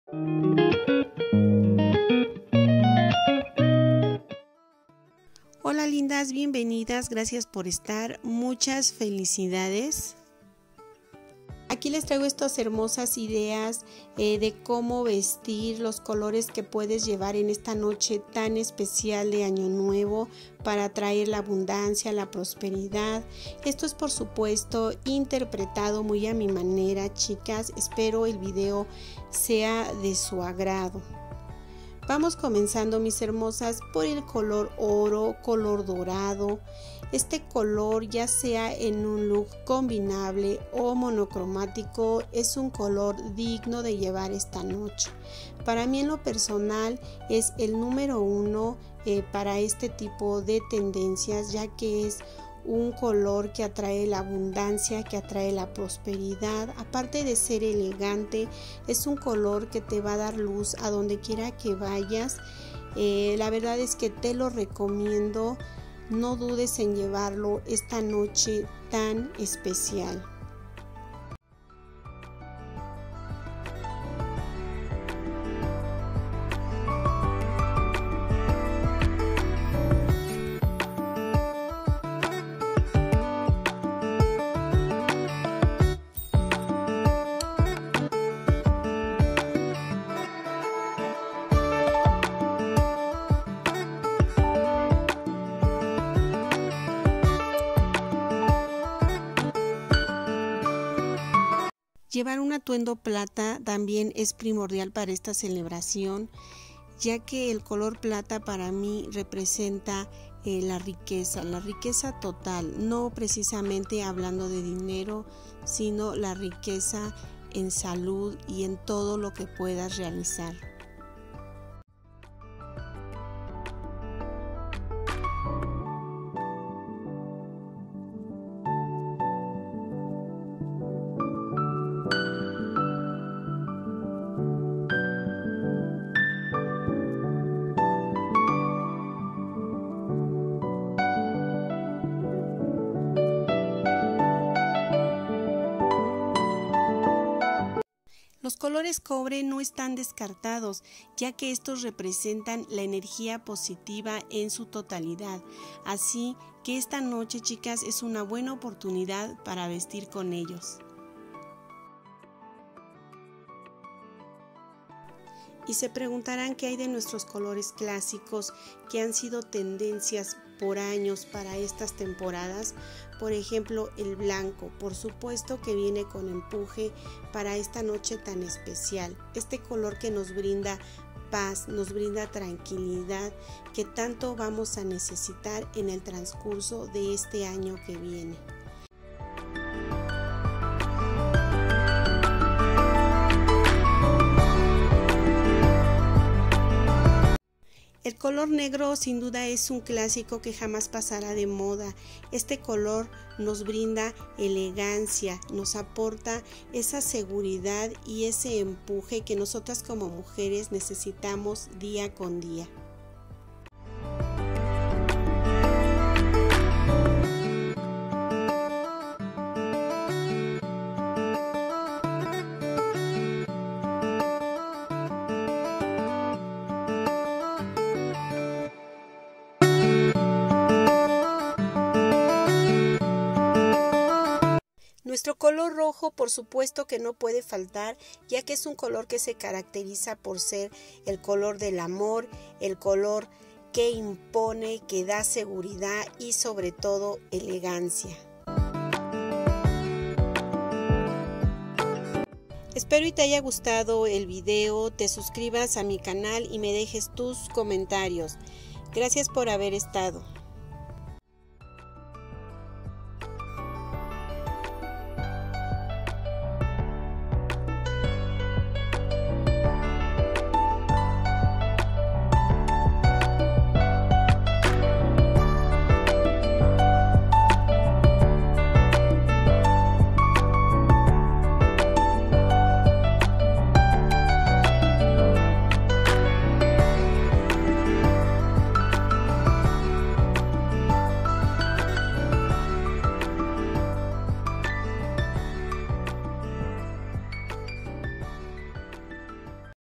COMO VESTIR PARA ATRAER PROSPERIDAD EN AÑO NUEVO 🌟 COLORES RECOMEMDADOS PARA CELEBRAR 🥂🎆
Hola mis hermosas 🌸 QUE ESTE AÑO QUE ESTÁ POR COMENZAR LLEGE CARGADO DE AMOR DICHA Y PROSPERIDAD 🎆🥂🌟🙏 Luce como una reina y celebra recibiendo el año nuevo con estos hermosos atuendos en colores de tendencia, exclusivos para esta noche tan especial.
Abundancia
Prosperidad
Salud
Tienes que ver 👌

Espero sea de tu agrado y me dejes tus comentarios y sugerencias 😊